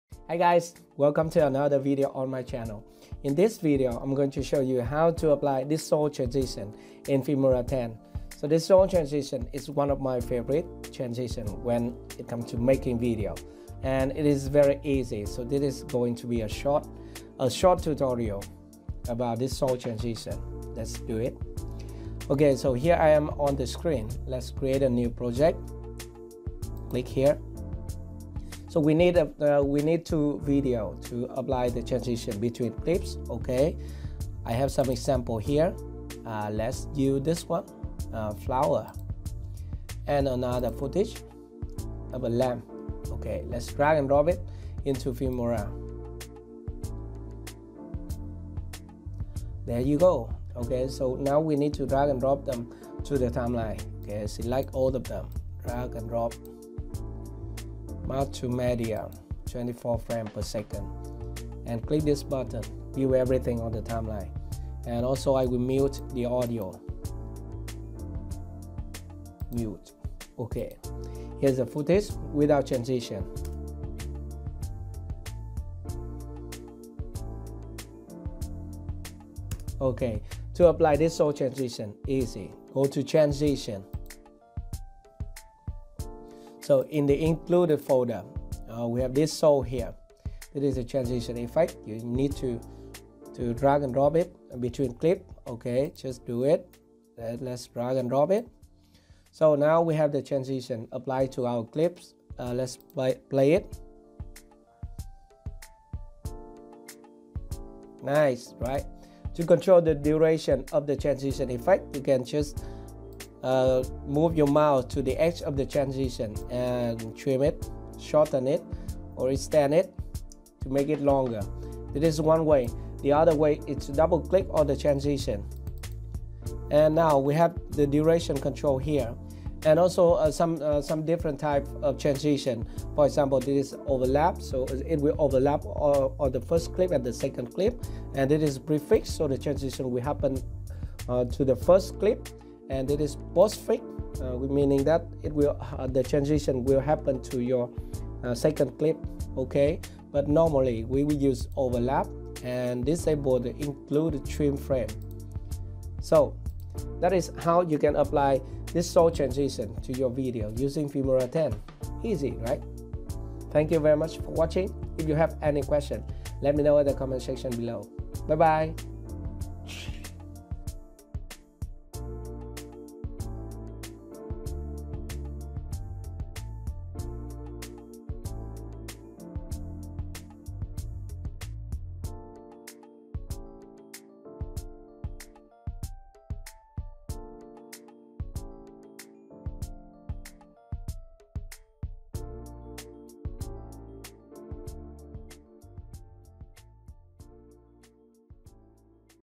Filmora X Dissolve Effect - How to Add Dissolve Transition to Video
This tutorial shows you how to add dissolve transition to your videos using Filmora X (or Filmora 10).